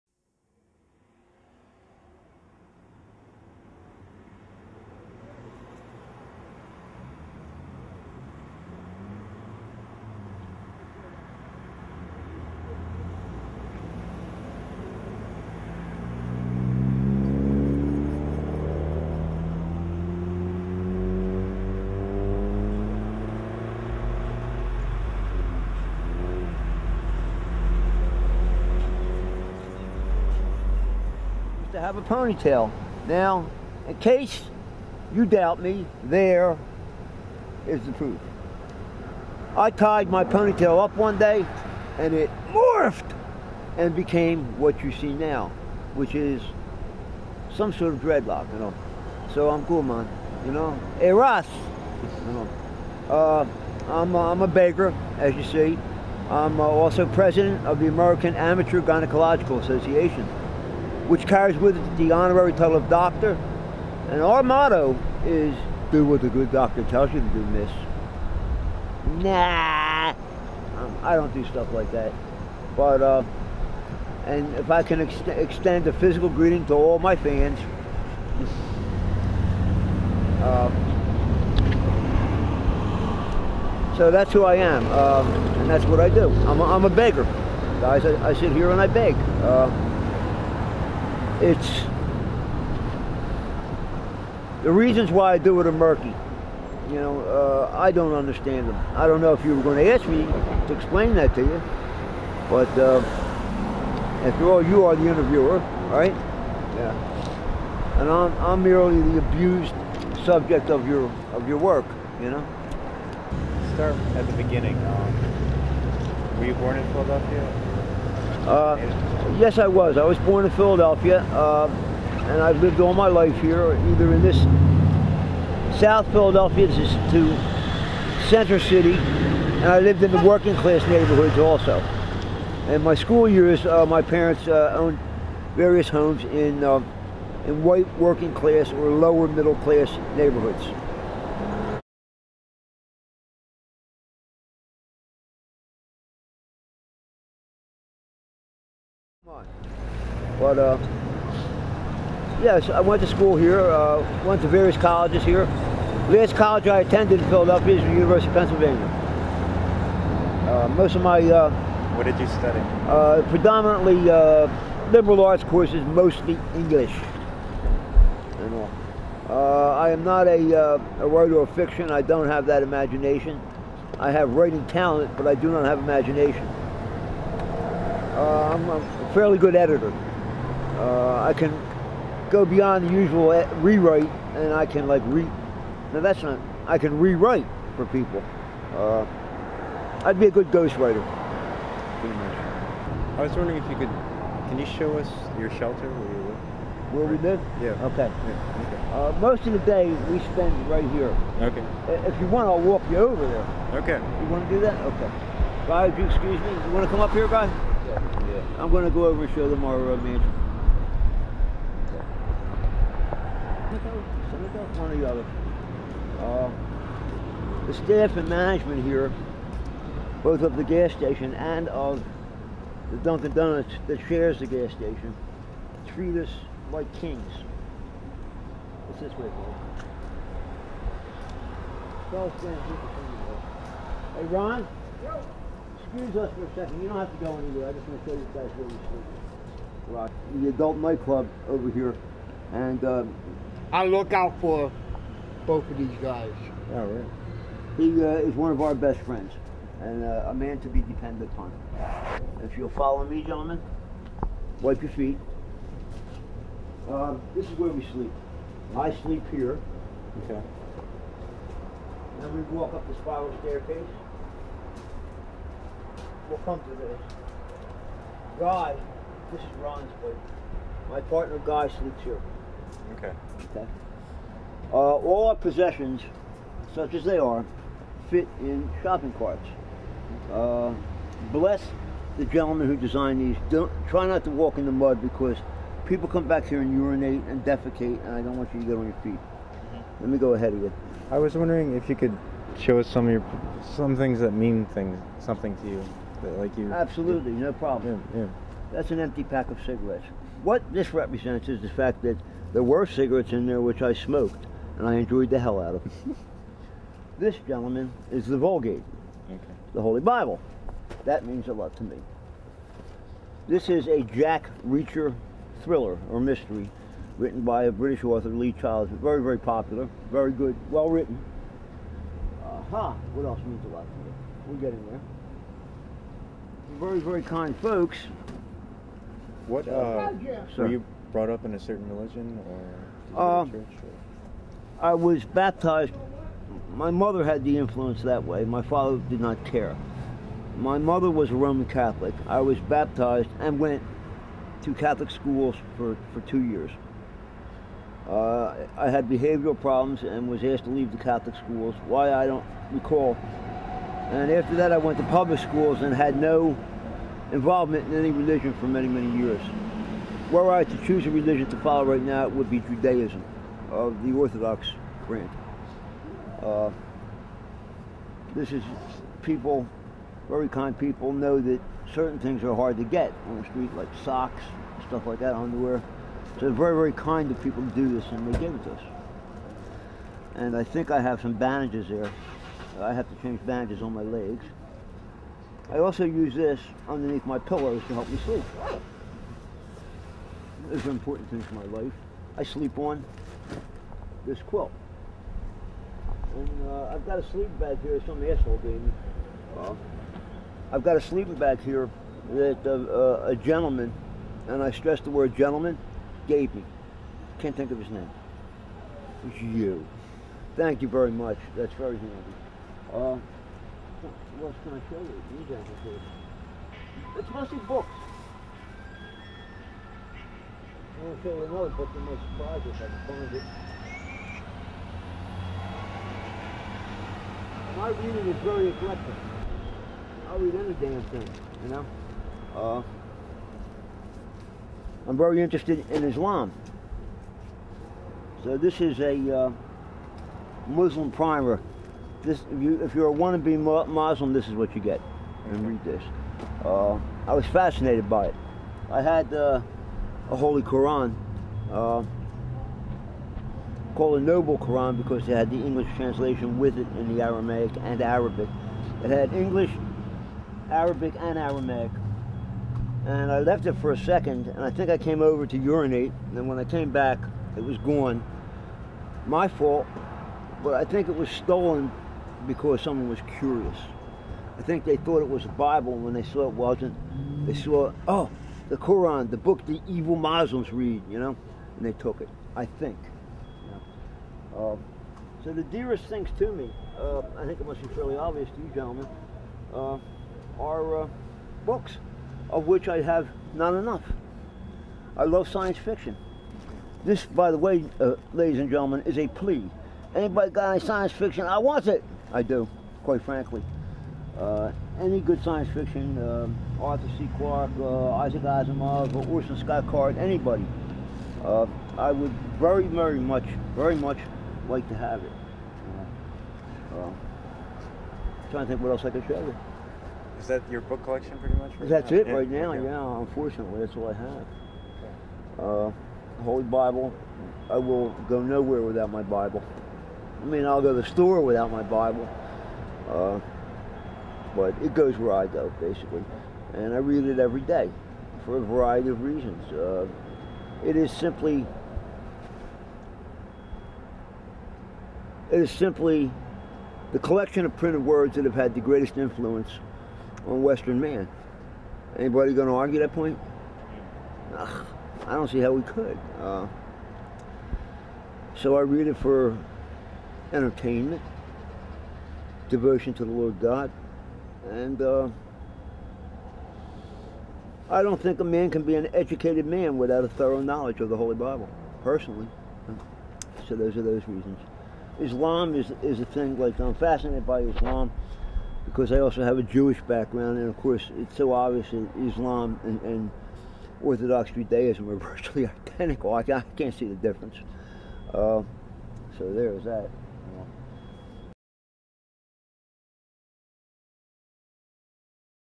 Charles Part 1
Unfinished rough cut interview with Charles of South Christopher Columbus Boulevard in Philadelphia, PA. Charles was homeless at the time of filming.